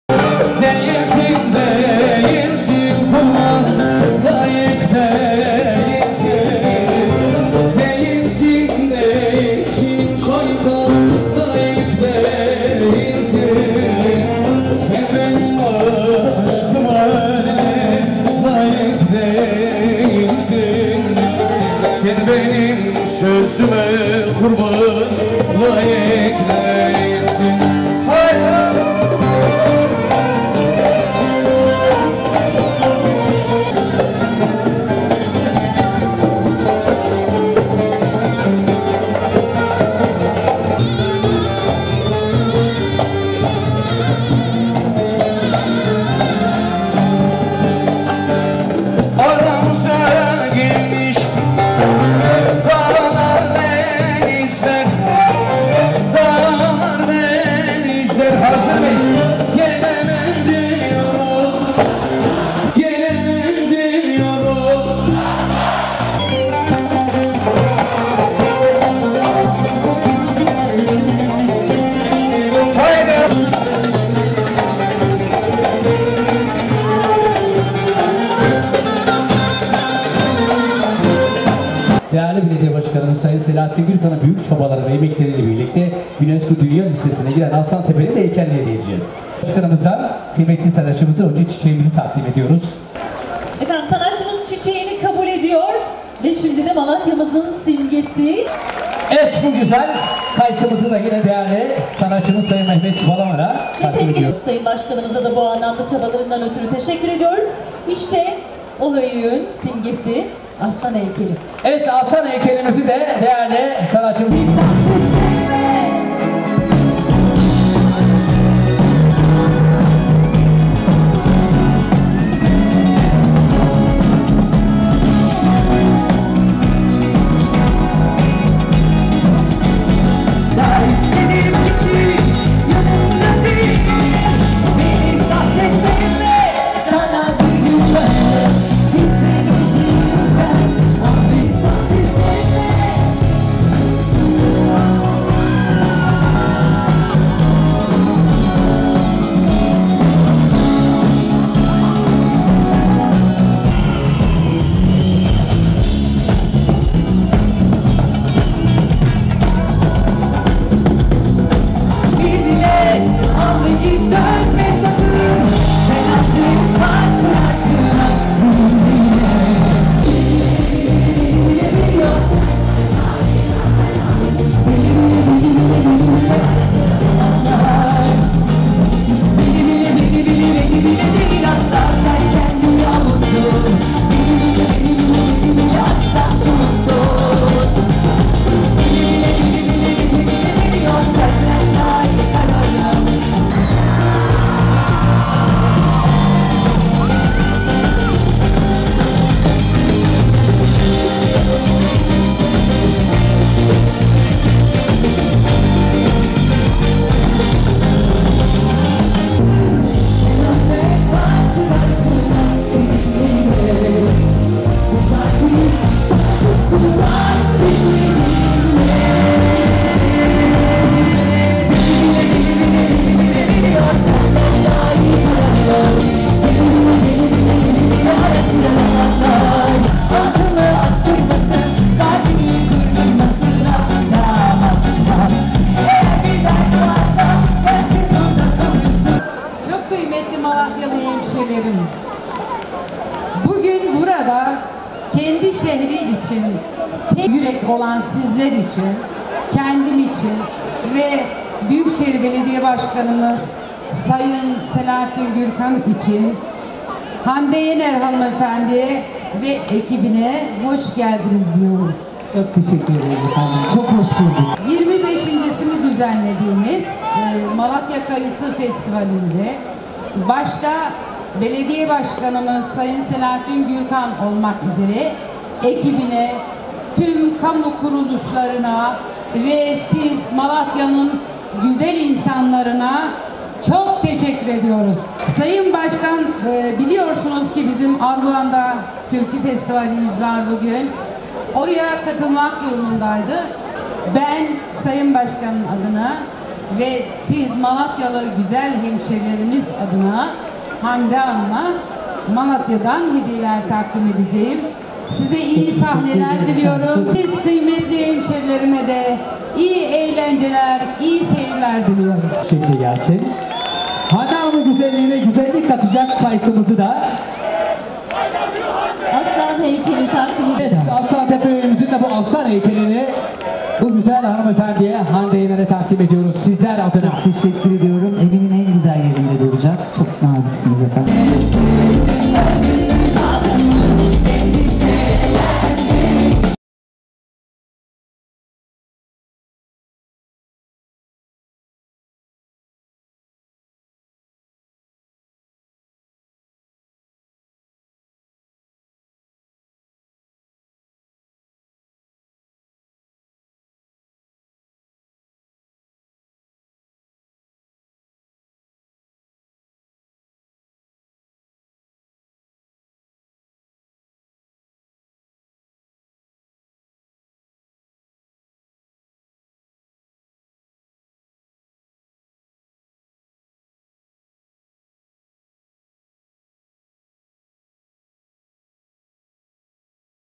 Hande Yener Kayısı Festivali Konseri
Mişmişpark Fuar alanında 25. Kayısı Festivalinin üçüncü gününde  Malatyalıların merakla ve heyecanla beklediği ünlü sanatçı Hande Yener sahne aldı. Hande Yener,  seslendirdiği birbirinden güzel eserlerle konser alanını dolduran on binlerce Malatyalıya unutulmaz bir gece yaşattı.